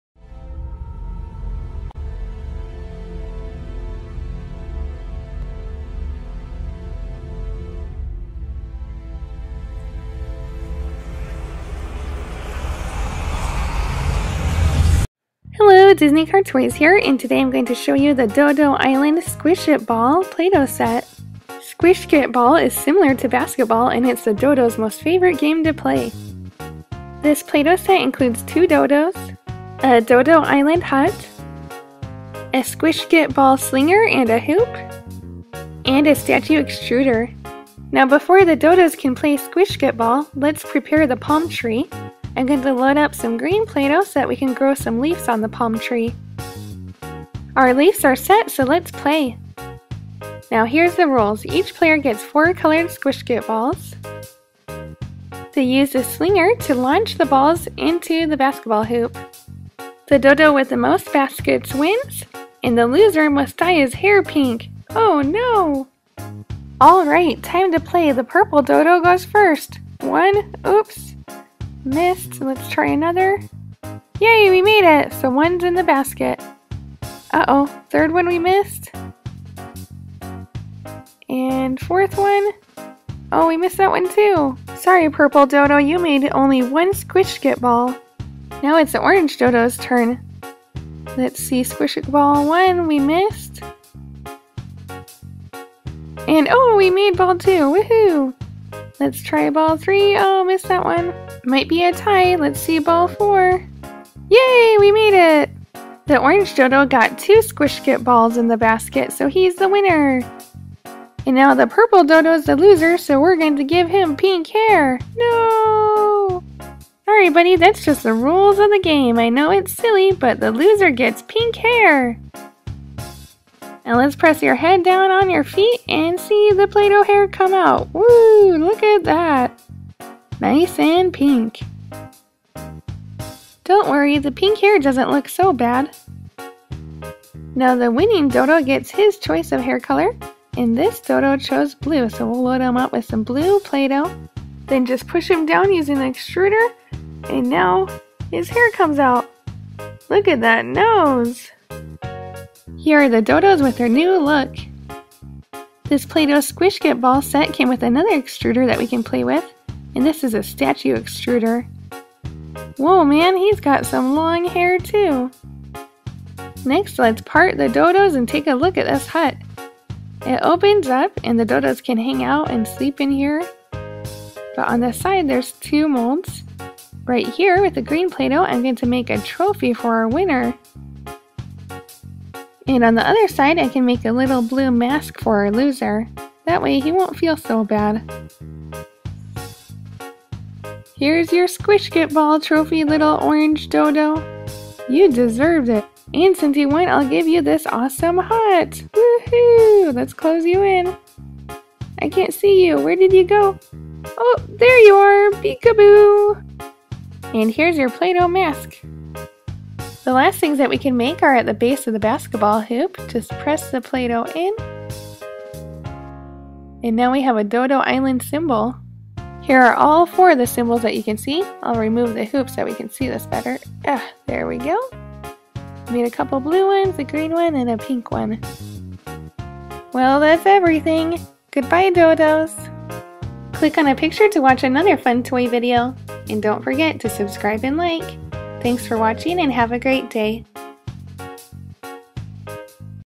Play Doh Doh Doh Island Squishketball Play Dough Basketball & Animal Hair Extruders DisneyCarToys
Play-Doh Doh Doh Island Squishketball set toy review by DisneyCarToys. The animals from Doh-Doh Island get together to play their favorite game, Squishketball, which is similar to a Basketball game. Make small play dough balls and launch them into the Play Doh Doh-Doh Island Squishketball Hoop. The Doh Doh Playdoh Animal that gets the most baskets wins, and the loser must get pink Play Dough Hair. The purple Doh Doh monster / animal lost and thus had to get pink play doh hair. To grow the pink playdoh hair, just load play doh in the extruder and push. Funny! This Play Dough Doh Doh Island Squishketball is part of the Doh Doh Island series, made in the early 2000s. So this set is slightly a vintage play dough set.